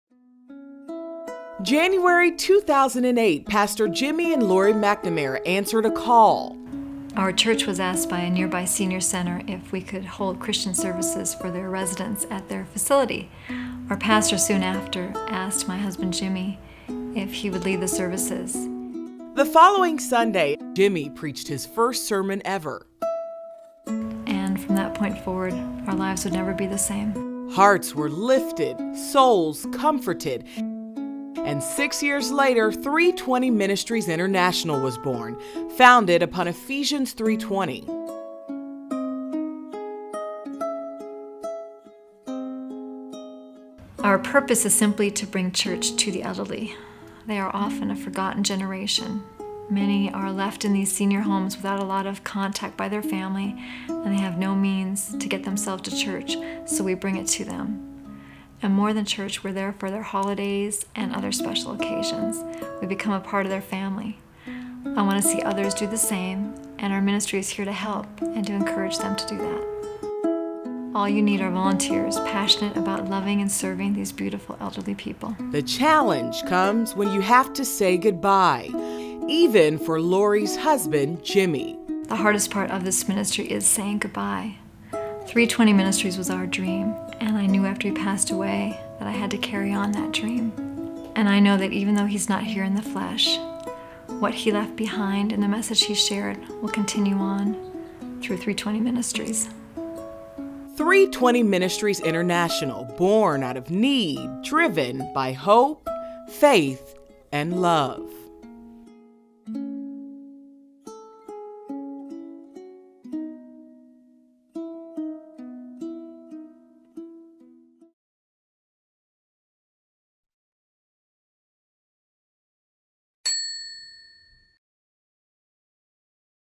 Reaching the Forgotten Generation with a Message of Hope - 320 Ministries International
The purpose and mission of 320 Ministries International is to bring awareness to the Christian community of the need and importance of bringing church to the elderly.